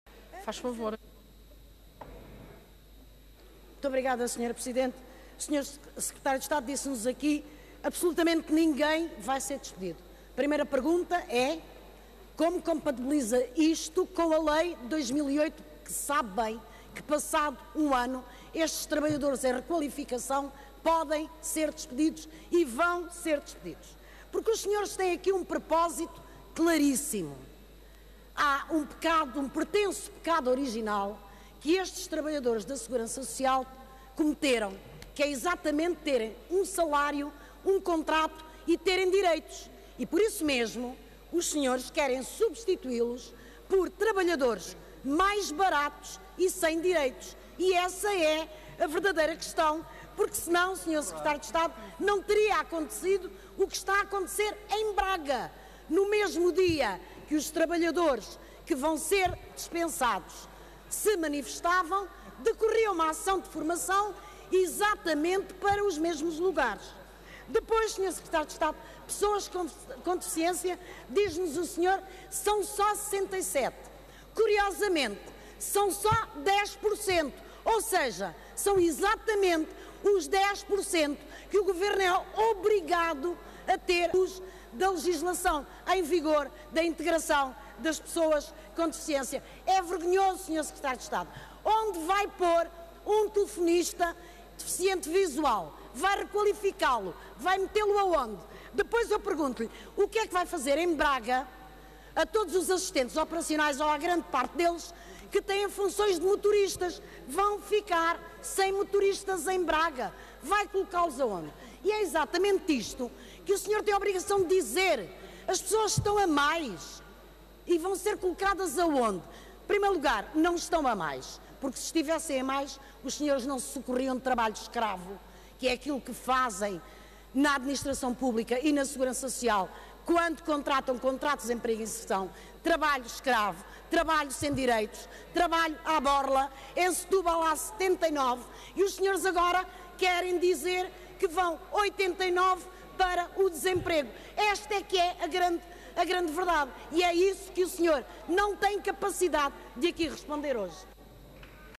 "Contratos Emprego Inserção são trabalho escravo"
Intervenção da deputada Mariana Aiveca no debate de atualidade sobre o despedimento de funcionários da Segurança Social. |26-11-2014|